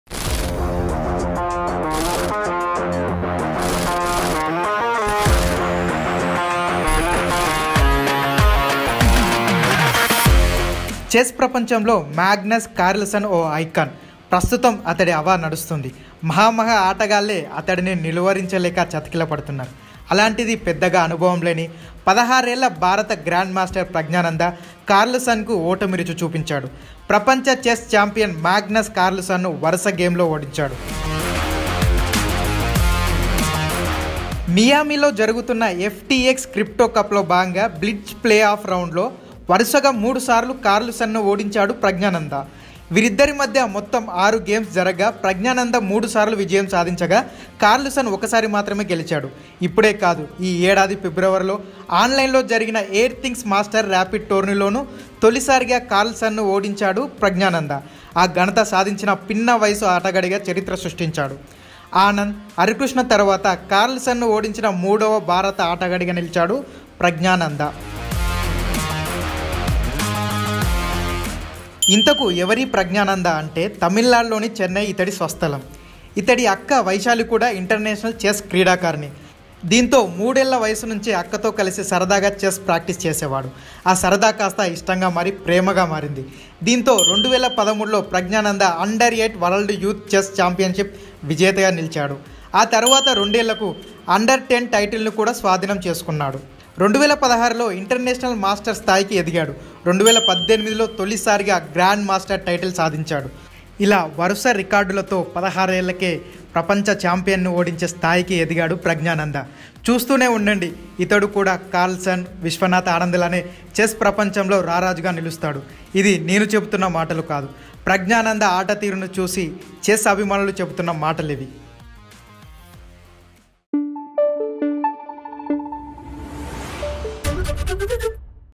Who is Grandmaster Praggnanandhaa : మాగ్నస్ కార్ల్ సన్ ను ఓడించిన చెన్నై చిన్నోడు | ABP Desam
Chess ప్రపంచంలో Magnus carlsen ఓ ఐకాన్. ప్రస్తుతం అతడి హవా నడుస్తోంది. మహా మహా ఆటగాళ్లే అతడిని నిలువరించలేక చతికిల పడుతున్నారు. అలాంటిది పెద్దగా అనుభవం లేని, 16 ఏళ్ల భారత Grandmaster  Praggnanandhaa .. కార్ల్‌సన్‌కు ఓటమి రుచి చూపించాడు. ప్రపంచ చెస్‌ ఛాంపియన్‌ మాగ్నస్‌ కార్ల్‌సన్‌ను వరుస గేమ్స్‌లో ఓడించాడు. మియామీలో జరుగుతోన్న
FTX క్రిప్టో కప్‌లో భాగంగా బ్లిట్జ్‌ ప్లే ఆఫ్‌ రౌండ్‌లో వరుసగా మూడుసార్లు కార్ల్‌సన్‌ను ఓడించాడు. వీరిద్దరి మధ్య మొత్తం ఆరు గేమ్స్‌ జరగ్గా.. ప్రజ్ఞానంద మూడు సార్లు విజయం సాధించగా.. కార్ల్‌సన్‌ ఒక సారి గెలిచాడు. ఇప్పుడే కాదు.. ఈ ఏడాది ఫిబ్రవరిలో ఆన్‌లైన్‌లో జరిగిన ఎయిర్‌థింగ్స్‌ మాస్టర్స్‌ ర్యాపిడ్‌ టోర్నీలోనూ తొలిసారి కార్ల్‌సన్‌ను ఓడించాడు. ఆ ఘనత సాధించిన పిన్న వయస్సు ఆటగాడిగా చరిత్ర సృష్టించాడు. Anand, Harikrishna తర్వాత కార్ల్‌సన్‌ను ఓడించిన మూడో భారత ఆటగాడిగా నిలిచాడు.